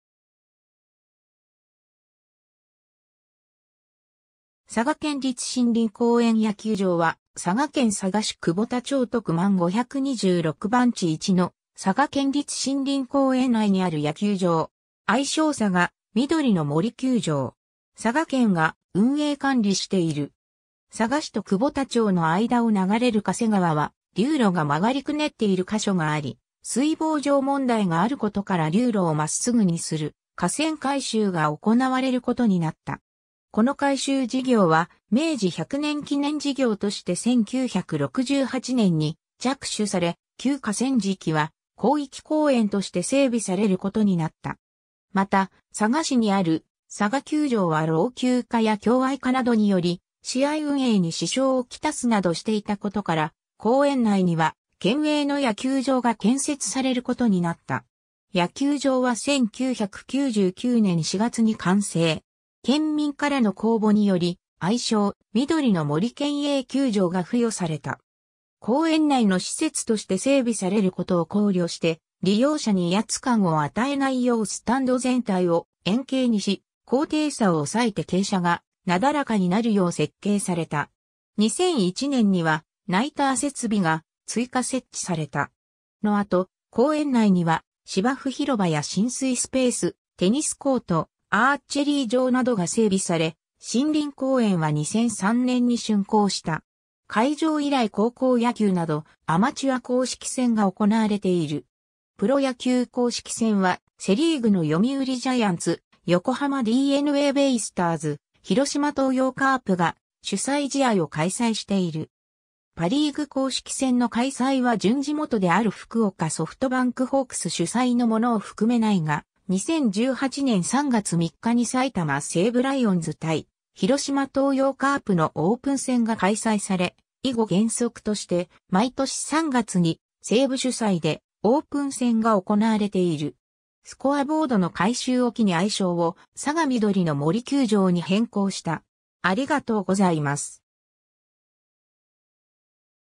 佐賀県立森林公園野球場
九州・沖縄地方の野球場
佐賀県立森林公園野球場（さがけんりつ・しんりんこうえん・やきゅうじょう）は、佐賀県佐賀市久保田町徳万526番地1の佐賀県立森林公園内にある野球場。
愛称 さがみどりの森球場（さがみどりのもり・きゅうじょう）。
佐賀県が運営管理している。
 佐賀市と久保田町（当時）の間を流れる嘉瀬川は流路が曲がりくねっている箇所があり、水防上問題があることから流路を真っすぐにする河川改修が行われることになった。
 この改修事業は明治百年記念事業として1968年に着手され、旧河川敷は広域公園として整備されることになった。
また佐賀市にある佐賀球場は老朽化や狭隘化などにより試合運営に支障をきたすなどしていたことから、公園内には県営の野球場が建設されることになった。
 野球場は1999年4月に完成。
県民からの公募により、愛称「みどりの森県営球場」が付与された。
公園内の施設として整備されることを考慮して、利用者に威圧感を与えないようスタンド全体を円形にし、高低差を抑えて傾斜がなだらかになるよう設計された。
2001年にはナイター設備が追加設置された。
の後、公園内には芝生広場や親水スペース、テニスコート、アーチェリー場などが整備され、森林公園は2003年に竣工した。
 開場以来高校野球などアマチュア公式戦が行われている。
プロ野球公式戦は、セ・リーグの読売ジャイアンツ、横浜DeNAベイスターズ、広島東洋カープが主催試合を開催している。
パ・リーグ公式戦の開催は準地元である福岡ソフトバンクホークス主催のもの（二軍はこの限りに非ず）を含め無いが、2018年3月3日に埼玉西武ライオンズ対広島東洋カープのオープン戦が開催され、以後原則として毎年3月に西武主催でオープン戦が行われている。
 スコアボードの改修を期に愛称を『さがみどりの森球場』に変更した。